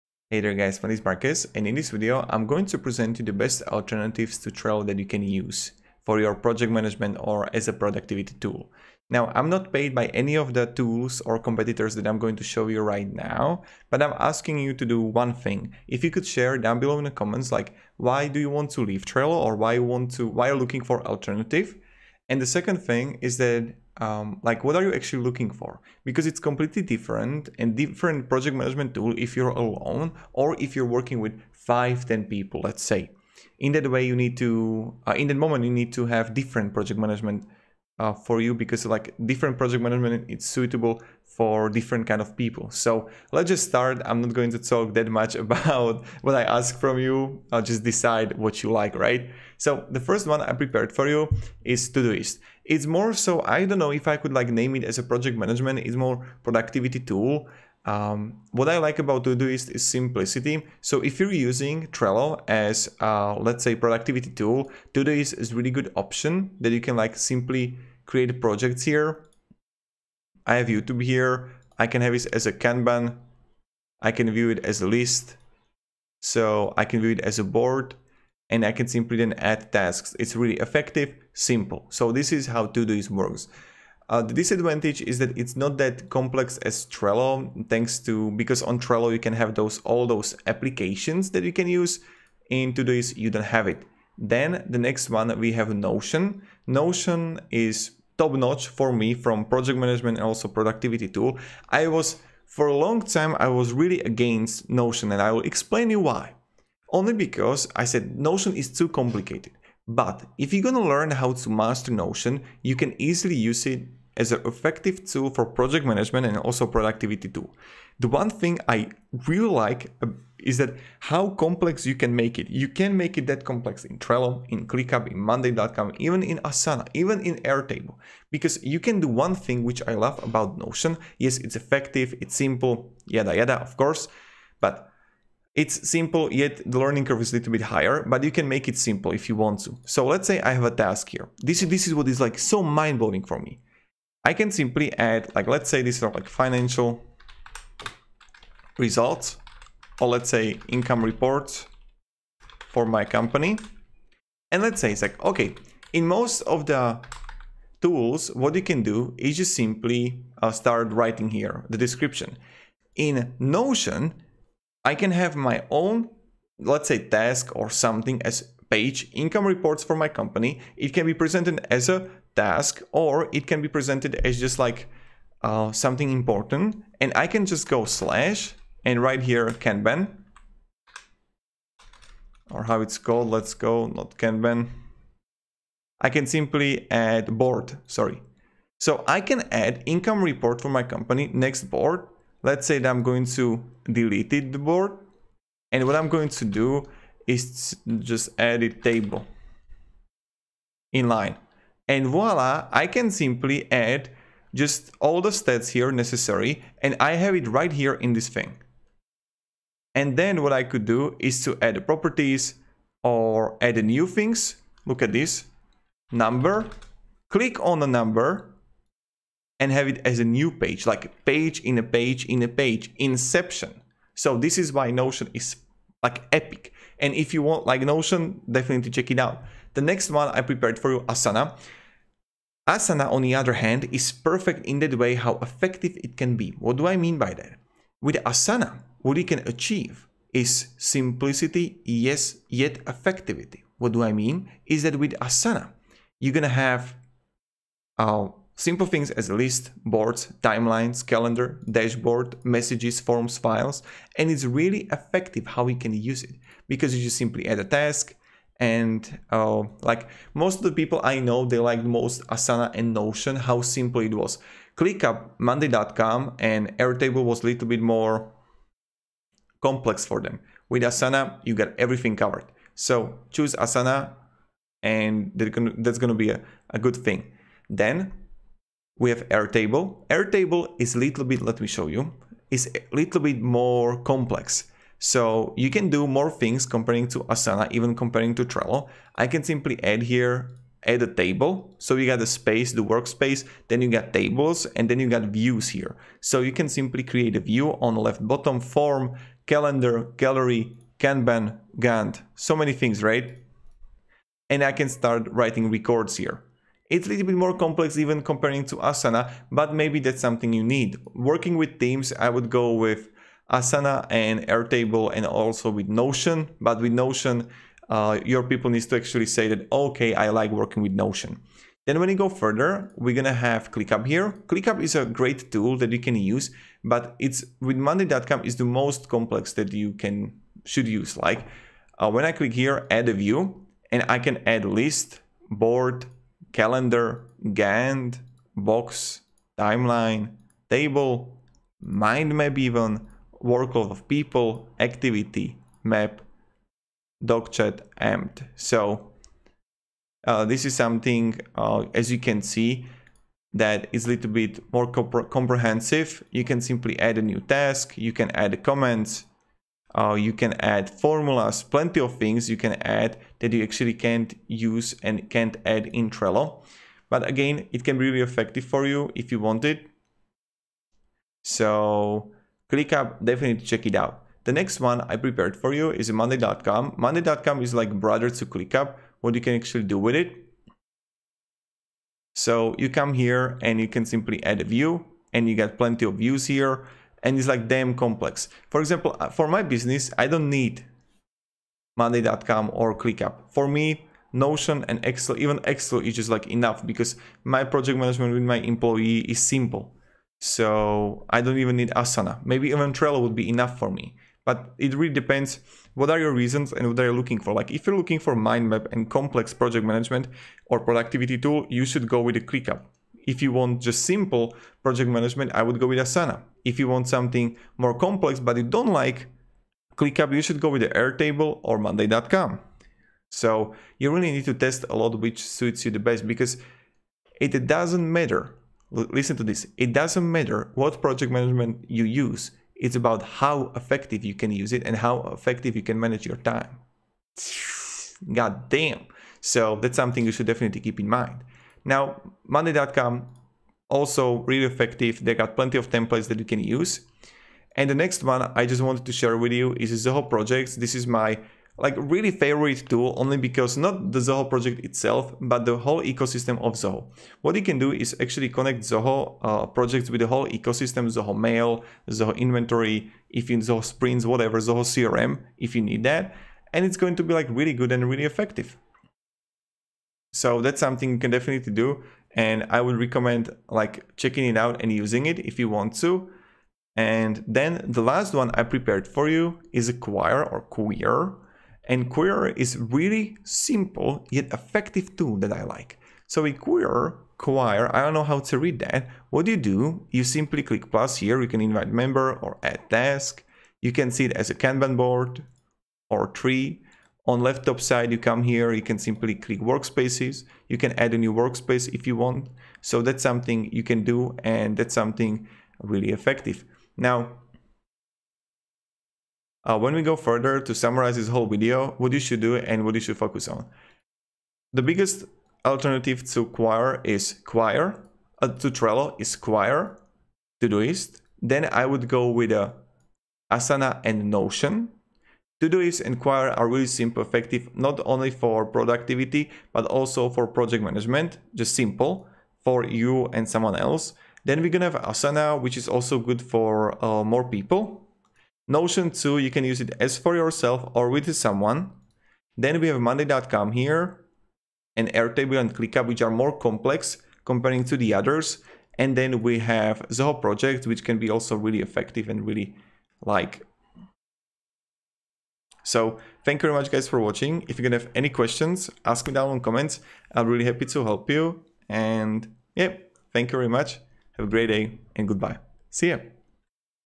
TOP 7 Trello Alternatives - Best Project Management Tools (2022)
In this video you will find out TOP 7 Trello Alternatives

GET AMAZING FREE Tools For Your Youtube Channel To Get More Views:
Tubebuddy (For GROWTH on Youtube):